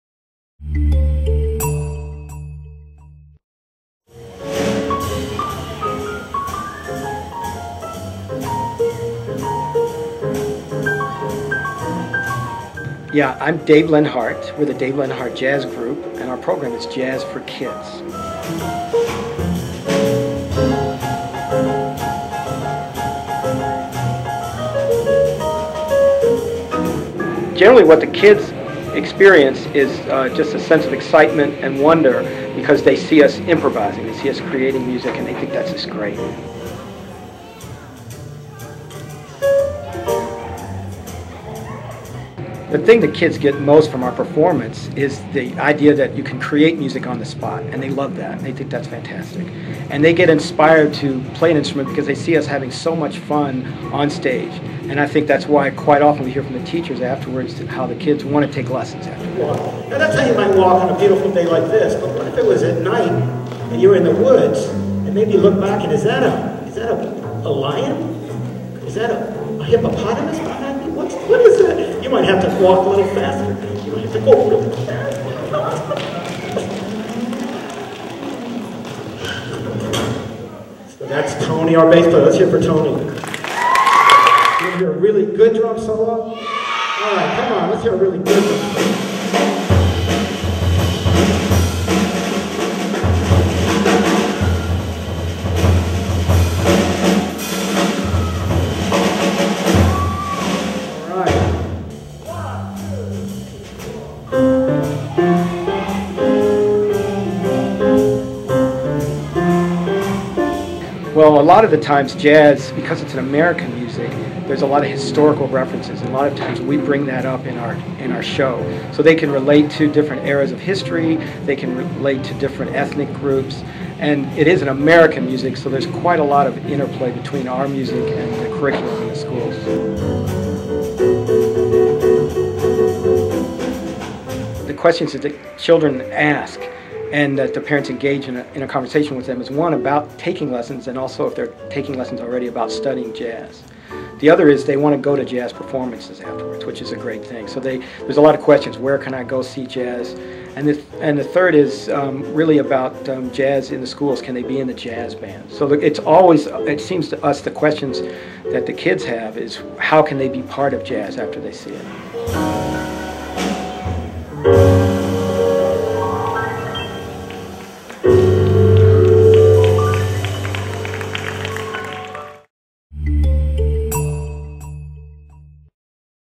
David Leonhardt "Jazz for Kids"
The fun, lively and exciting JAZZ FOR KIDS performance / demonstration acquaints children with concepts, instruments and styles of jazz and involves audience participation through singing and percussion.  Using familiar music selections, the jazz ensemble introduces a range of jazz concepts, with a strong focus on the improvisational aspects of the form.